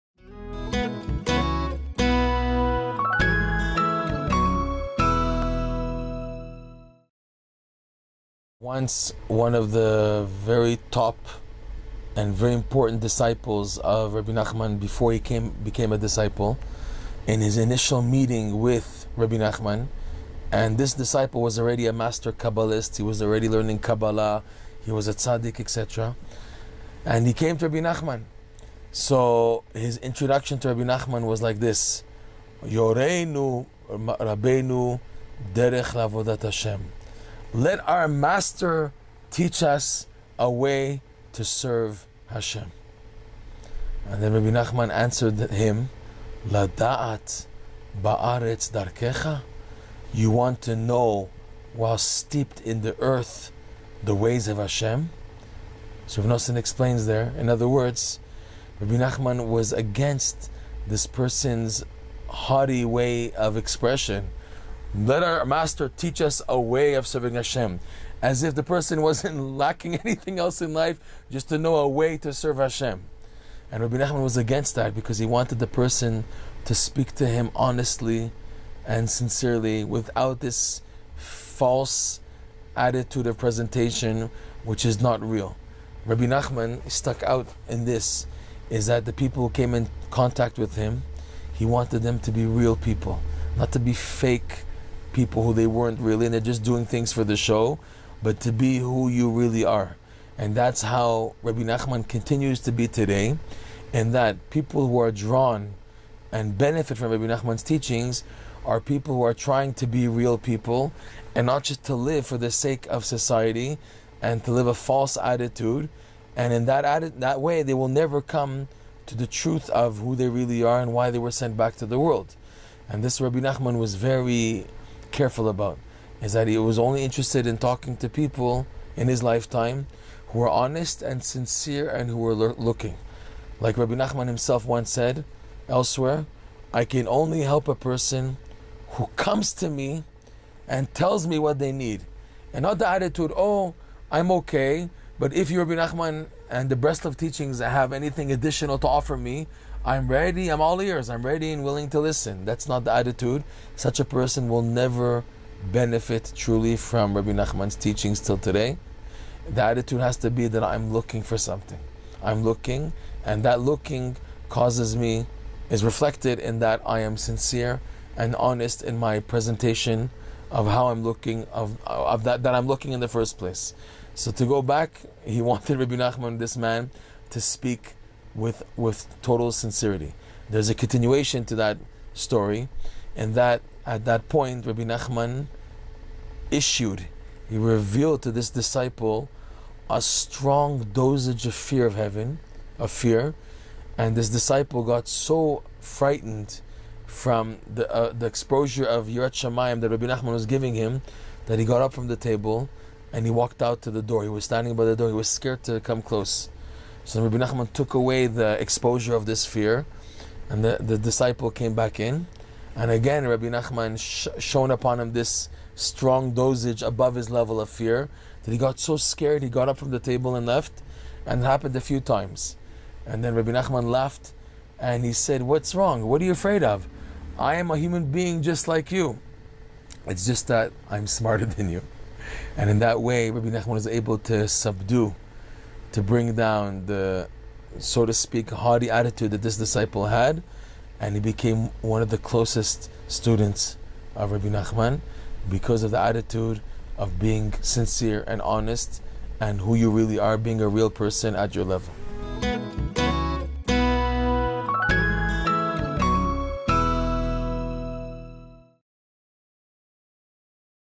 The Rebbe & The Kabbalist
Once an arrogant, master kabbalist came to Rebbe Nachman...in this short story of a meeting between Rebbe Nachman and a learned but haughty man, Meir Elkabas explains the importance of honesty, humility, and seeking the truth.